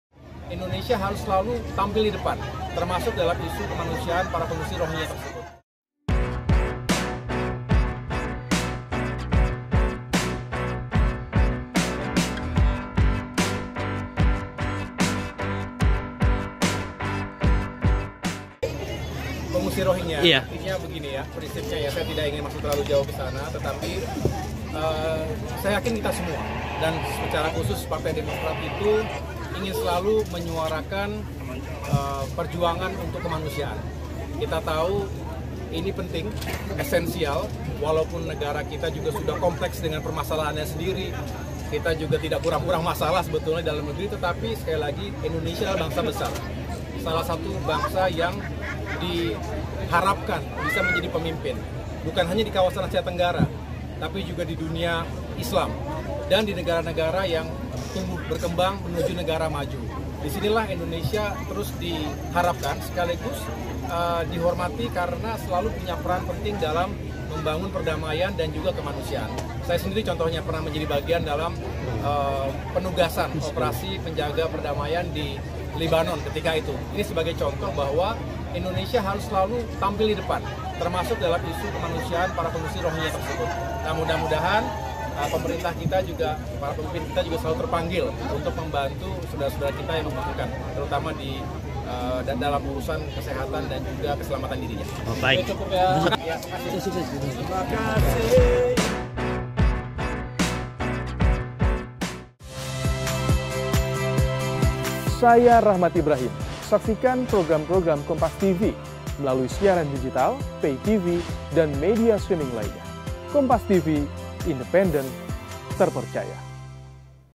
Soal Pengungsi Rohingya yang ke Indonesia, Ini Kata Ketum Demokrat AHY
KARAWANG, KOMPASTV - Ketua Umum Partai Demokrat Agus Harimurti Yudhoyono angkat bicara terkait penanganan pengungsi Rohingya yang terus berdatangaan ke Indonesia.

AHY berpendapat bahwa Indonesia bisa tampil dan berperan dalam penanganan pengungsi Rohingya.

"Prinsipnya saya yakin kita semua Partai Demokrat menyuarakan untuk kemanusiaan, ini penting essential. Indonesia bangsa besar salah satu bangsa yang bisa jadi pemimpin, di Asia Tenggara, di dunia islam, di sinilah Indonesia diharapkan dihormati punya peran penting bangun perdamaian dan kemanusiaan," kata AHY, di Karawang, Senin (11/12/2023).